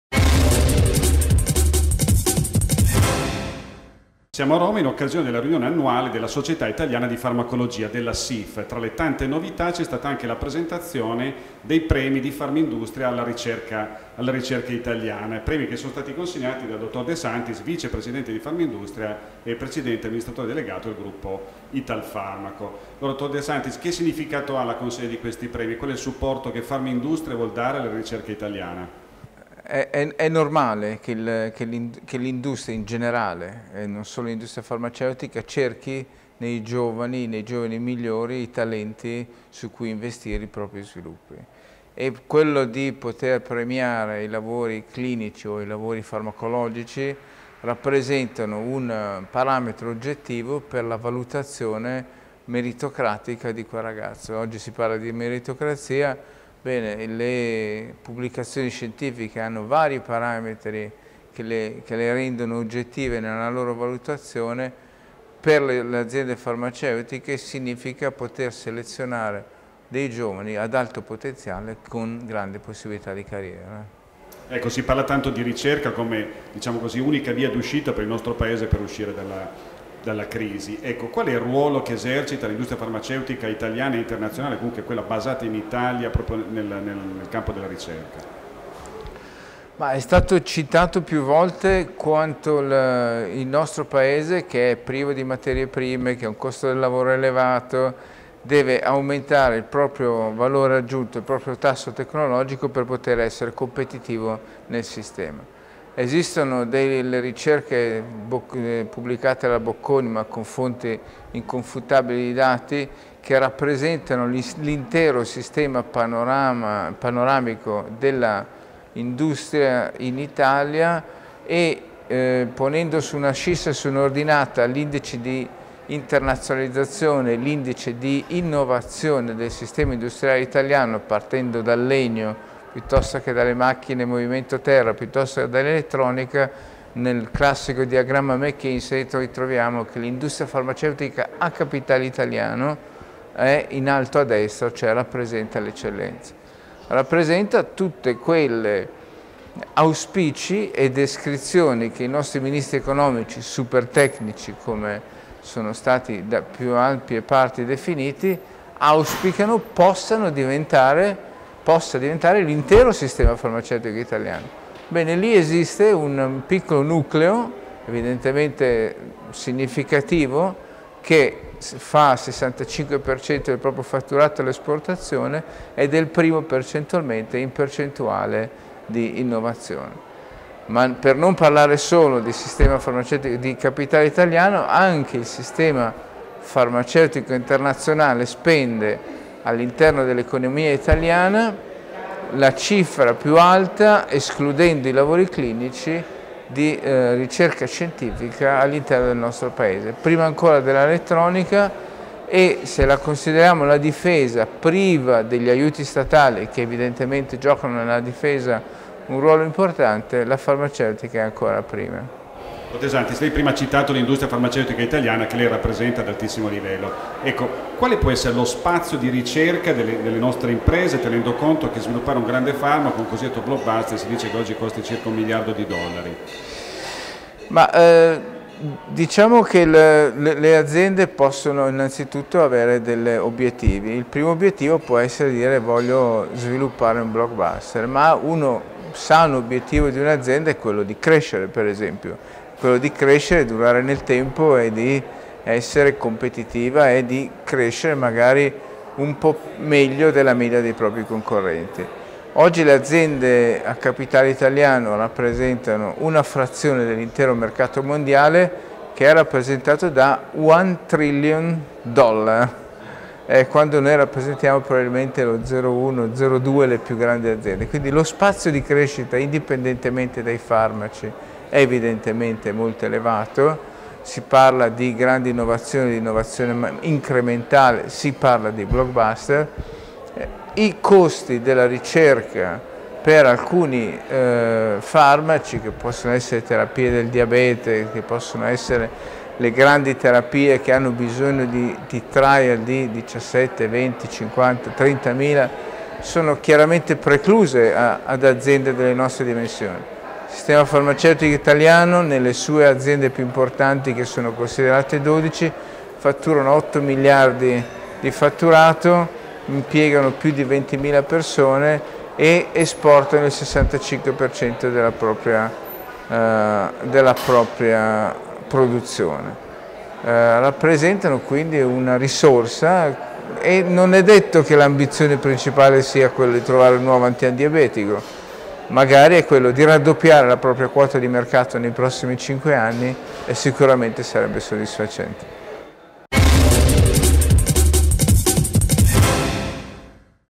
La ricerca farmaceutica in Italia
Intervista a Dr. Francesco De Santis presidente Italfarmaco e vice presidente Farmindustria